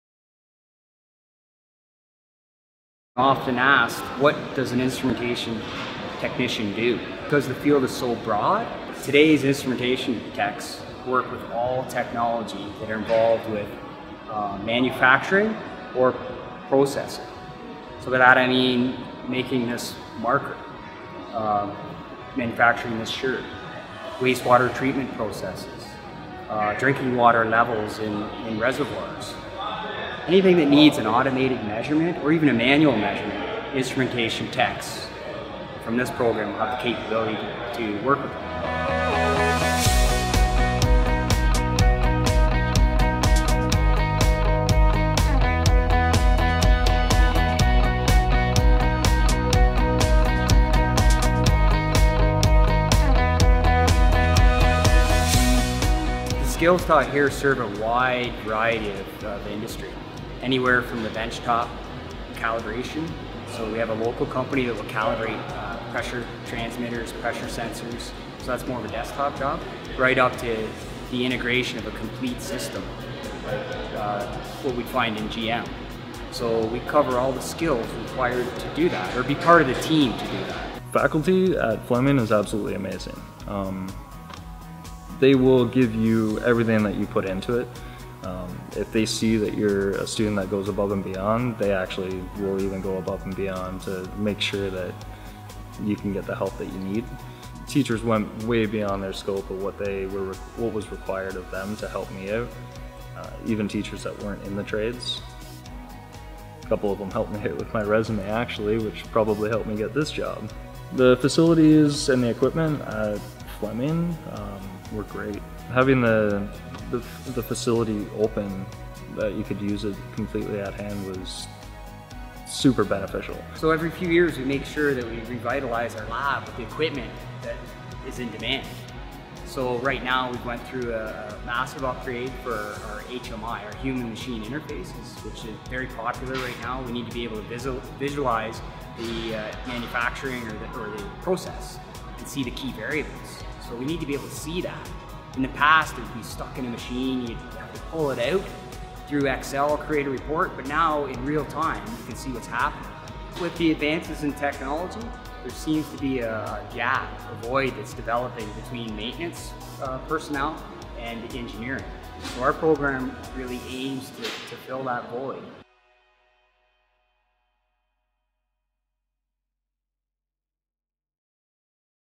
Instrumentation Control and Engineering Technician program Fleming College
The program coordinator describes the variety of careers that the Instrumentation Control and Engineering Technician program leads to, and the skills that students acquire.